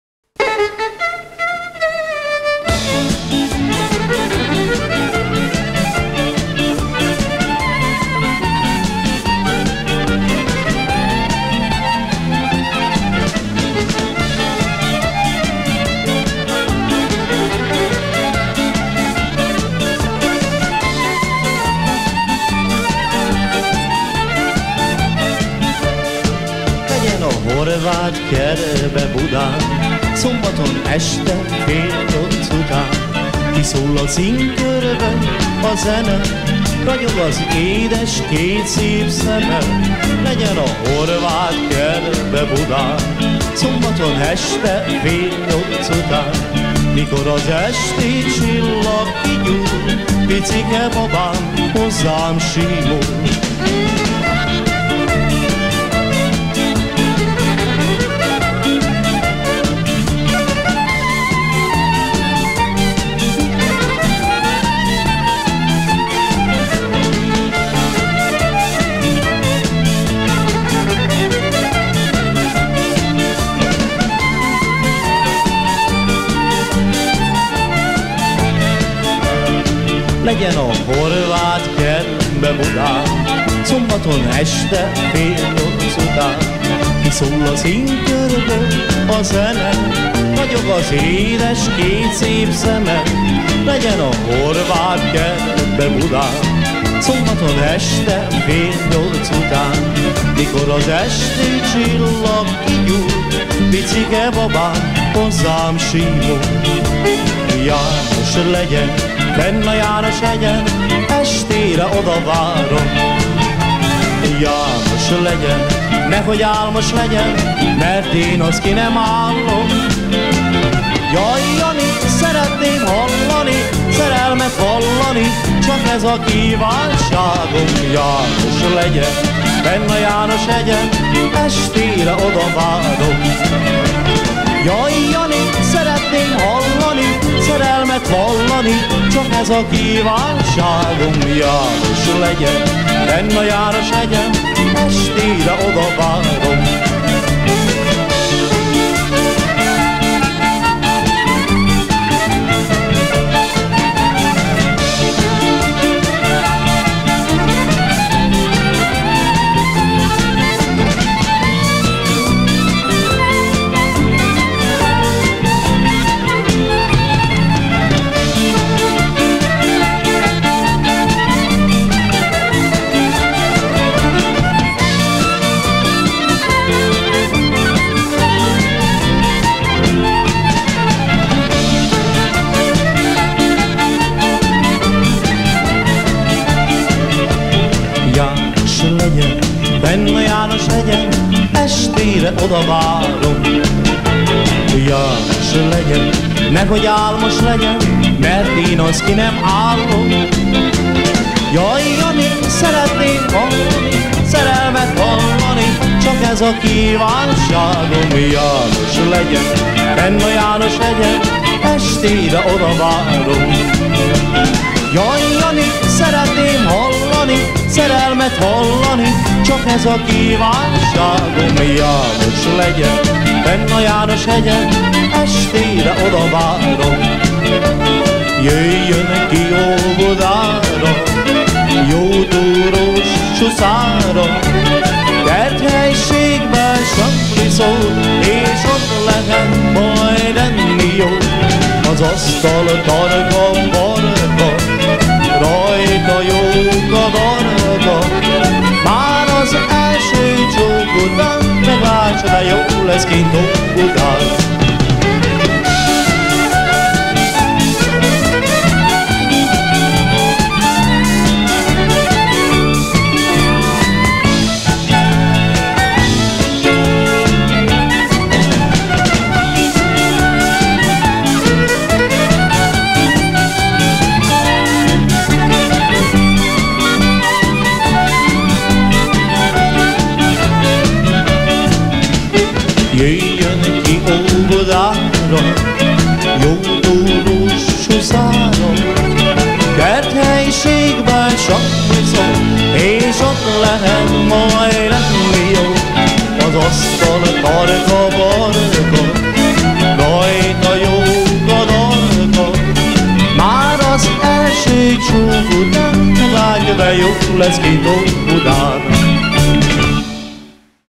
3+2 együttes - Nótaegyveleg - Győr, 1987
Győr, 1987

3+2 együttes
Bugyi Zoltán - ének, gitár
Bugyi Gyula - gitár
Bugyi János - dob
Góbor Attila - billentyűs hangszerek
Spör Helmut - basszusgitár

Burai Károly - billentyűs hangszerek, klarinét
Schőn Ignác - hegedű